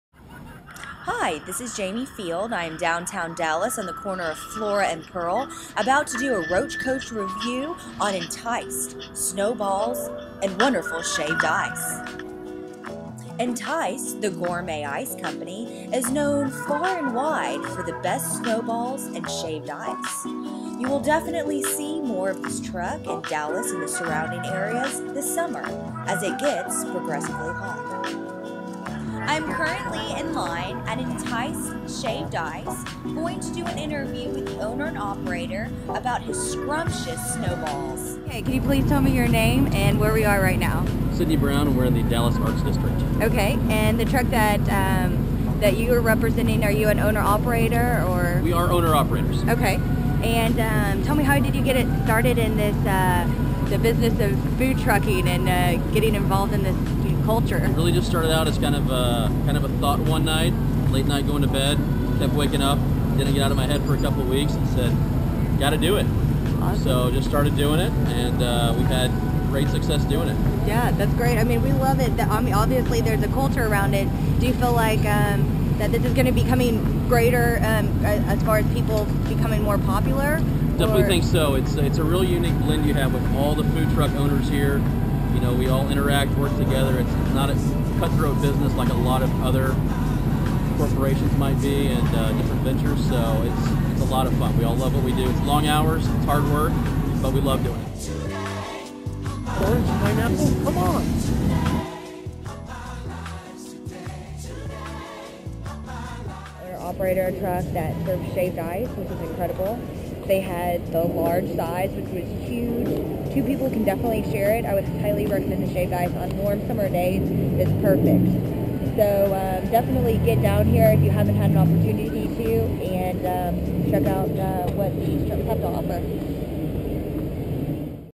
Entice
Roach Coach Review of Dallas food truck Entice with shaved ice and snow balls